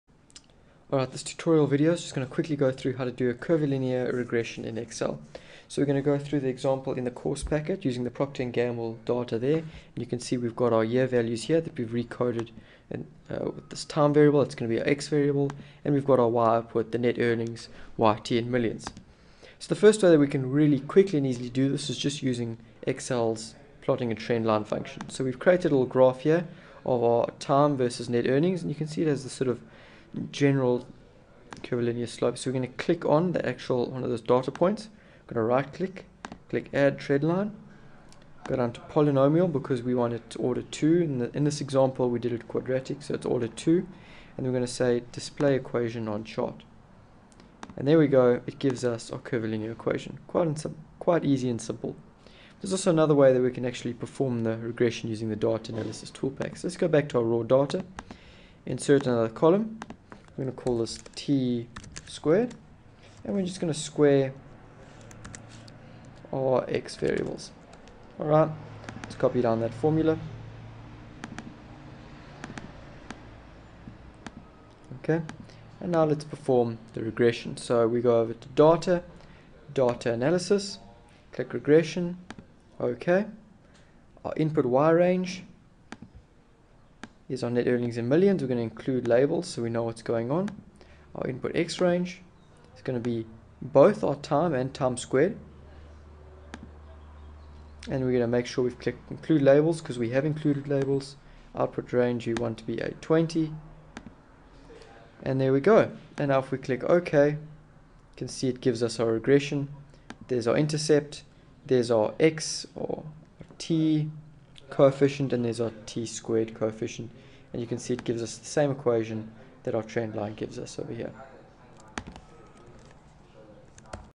Curvilinear Trend Model Example
From pg. 207 in the course packet.

To follow along, download the excel workbook from Blackboard.